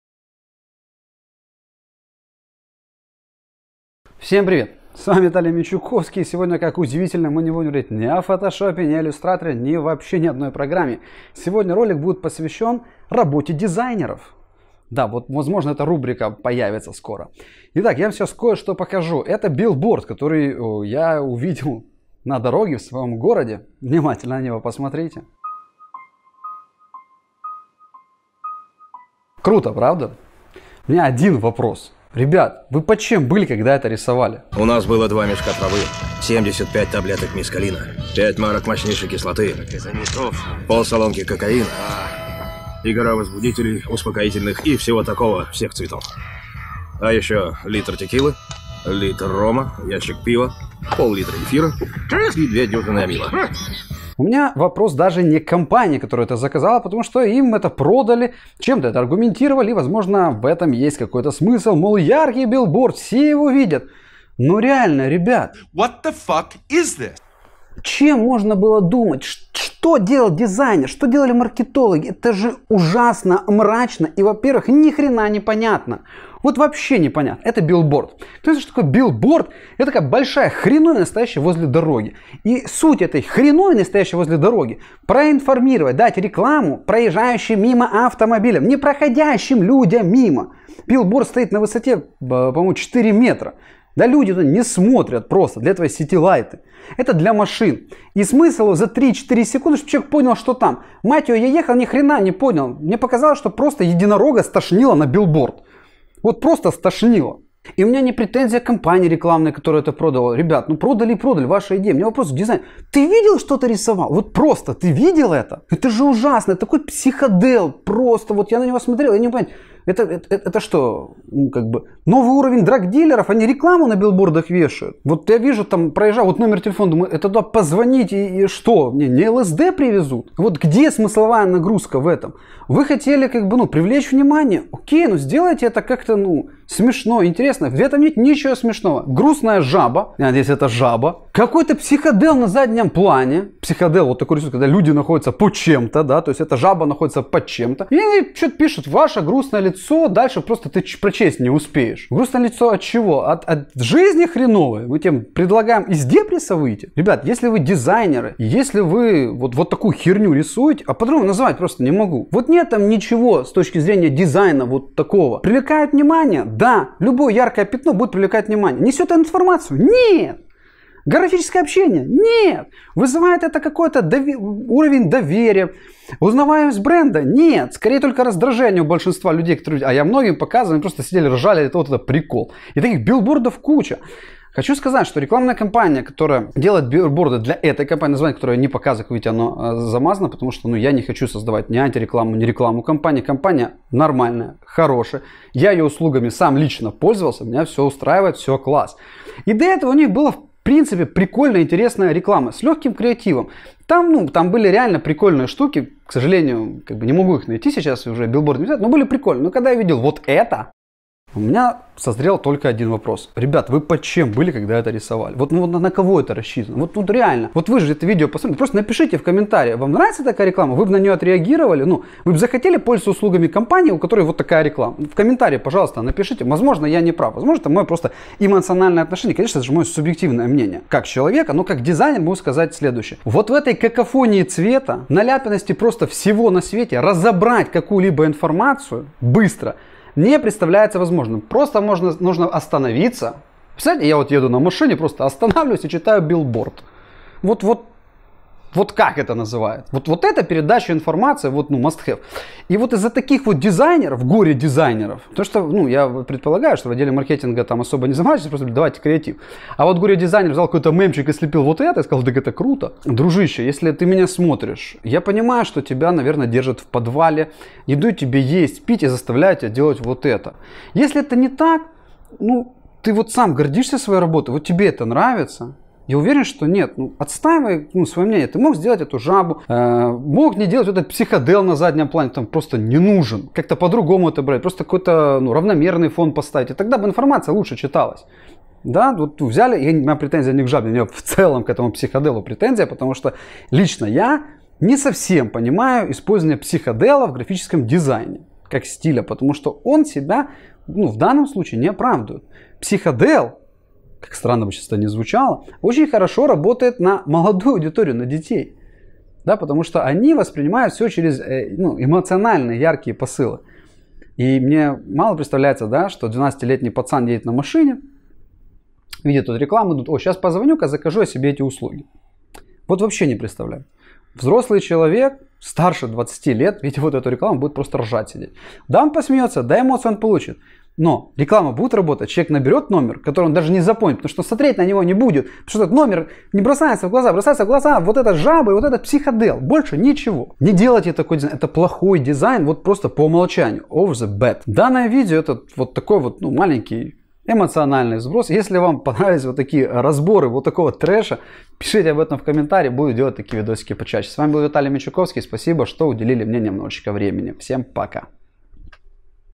😱 ДИЗАЙНЕР УРОВНЯ БОГ 😂
Я таки нашел лучшего дизайнера в мире! Это трешь который стоит посмотреть!